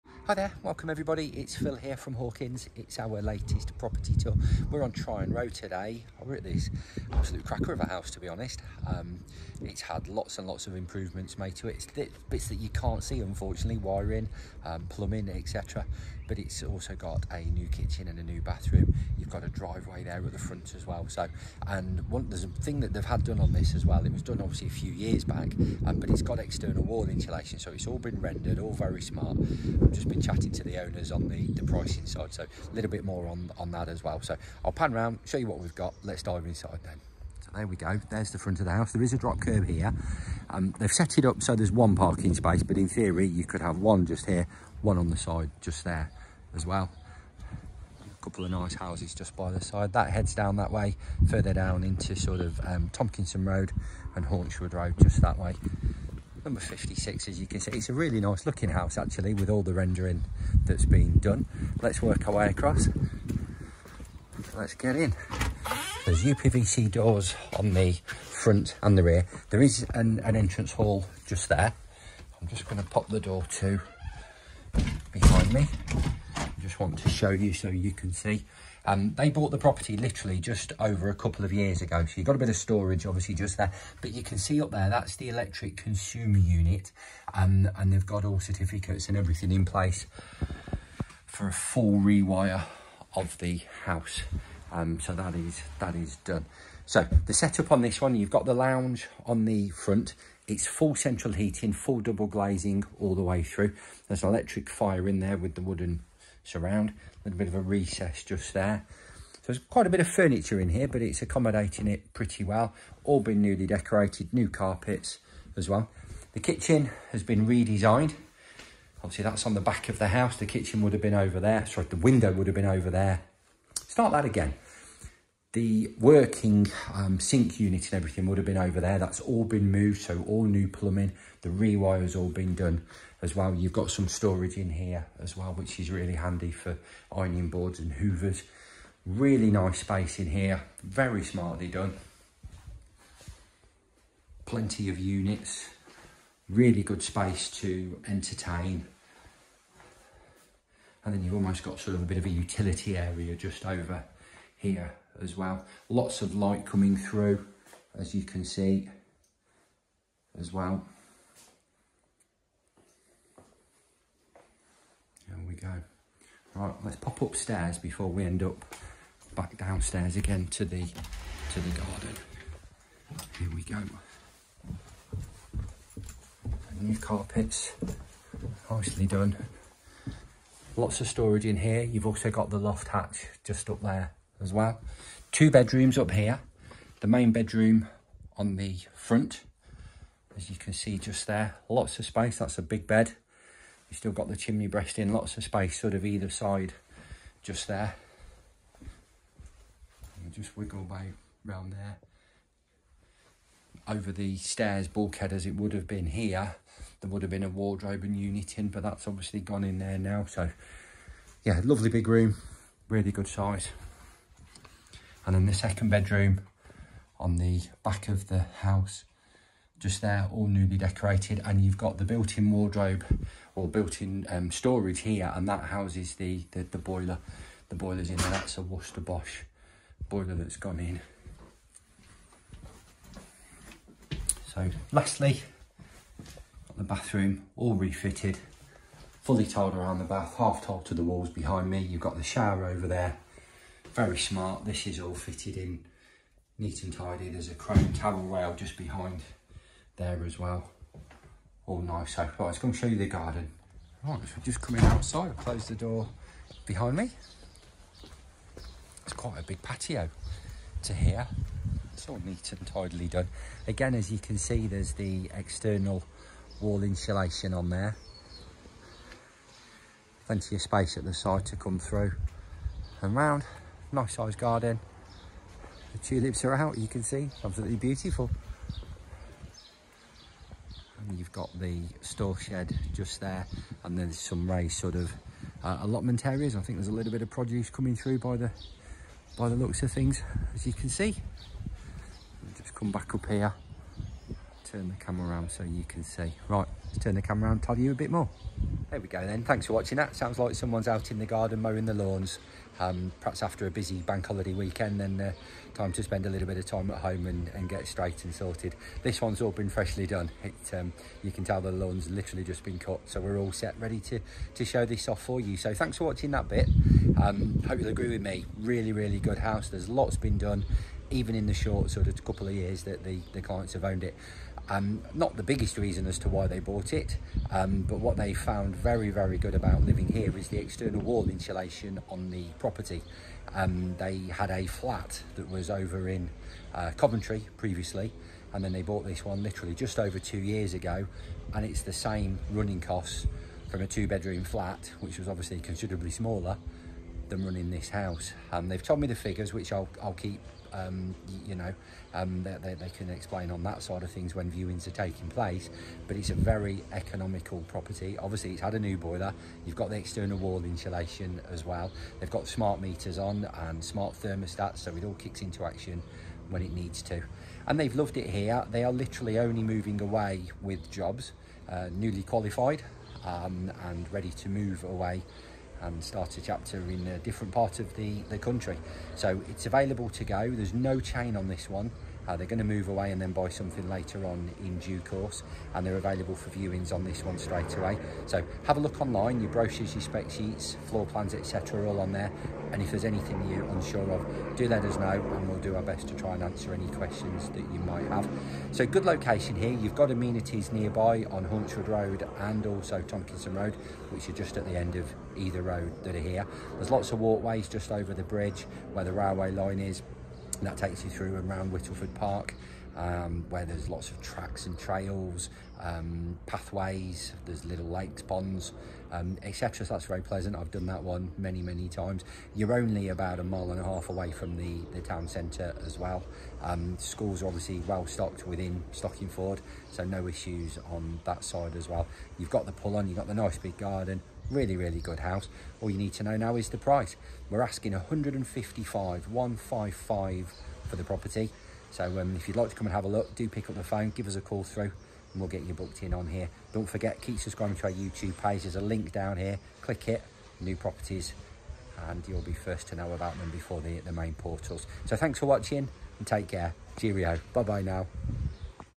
Property for sale in Nuneaton - Tryan Road Stockingford with Hawkins Estate Agents
*** TRY THIS ONE OUT ? *** Here is a vastly improved and well presented end terraced residence situated in this established location just off Tomkinson / Haunchwood Road.

The property offers comfortable and well planned accommodation of good sizes throughout with gas fired central heating, upvc double glazing, external wall insulation, car draw on to the front, long rear garden and would suit somebody looking to get onto the property ladder or an excellent rental investment with rents achieved nearby circa £650pcm.

Briefly comprising: entrance hall, lounge with feature fireplace, refitted / redesigned dining kitchen with built in oven and hob, landing, two good sized bedrooms and newly fitted bathroom. Car pull on and good sized rear garden. EPC RATING TBC.